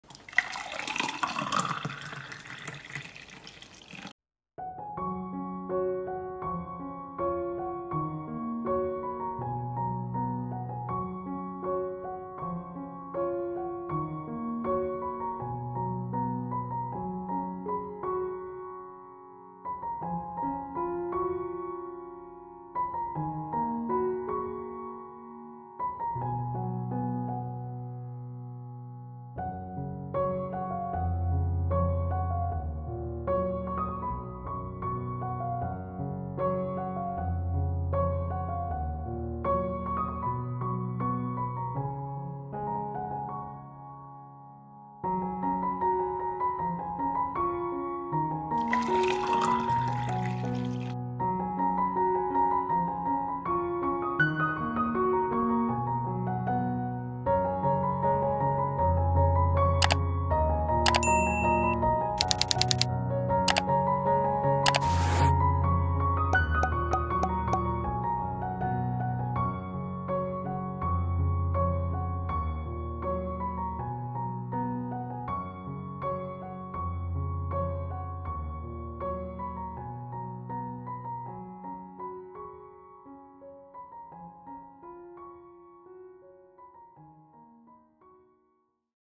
Anti-inflammatory Drink | Boost your Immune System
The best anti-inflammatory drink recipe so easy, healthy, tasty and refreshing. This anti-inflammatory drink recipe is a natural way to fight inflammation and boost your immune system.

Slow juicer
Cutting board

Ingredients
Clementines - 9 medium-sized
Ginger -2 thumb-sized